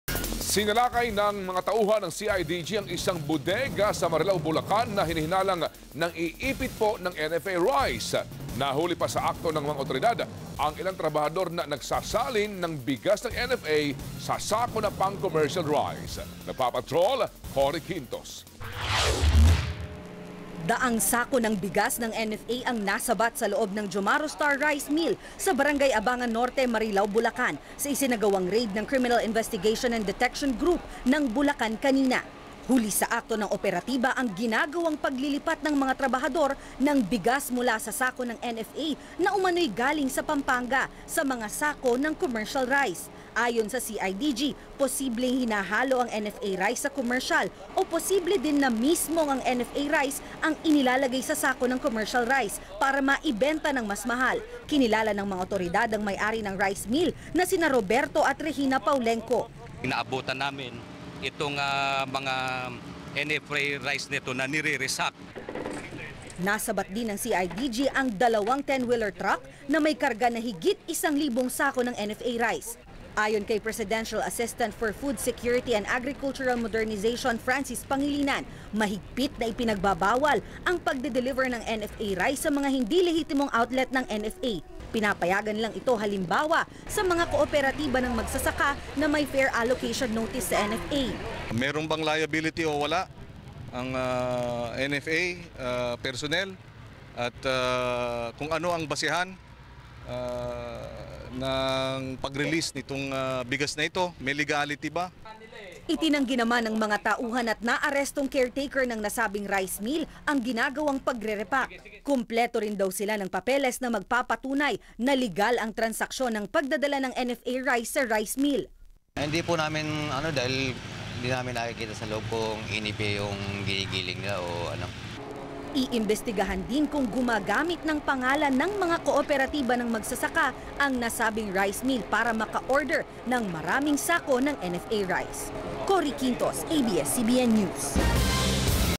Warehouse suspected hoarding of NFA rice raided
Presidential Assistant for Food Security Francis Pangilinan confers with members of the PNP Criminal Investigation and Detection Group (CIDG) who raided a warehouse in Marilao, Bulacan. The establishment is suspected of repacking NFA rice for commercial distribution.